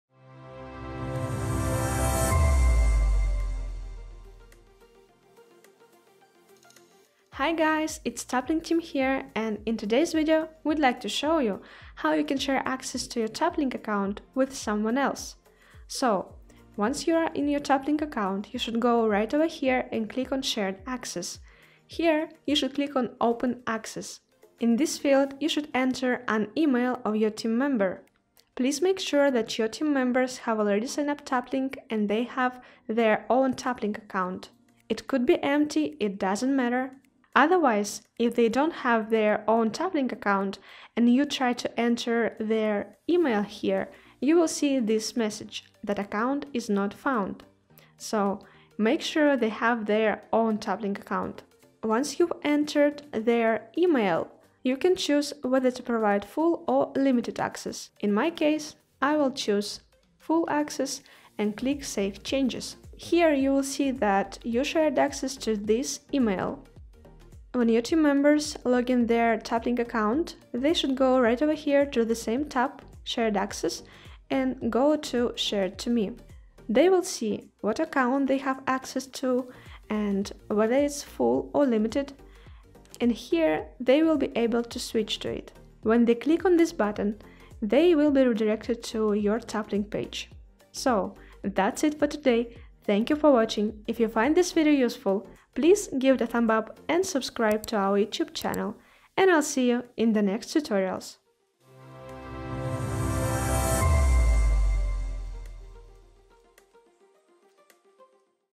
Taplink Tutorial: How to share access to your account?
In this tutorial, you will learn how you can share access to your Taplink account with your team members.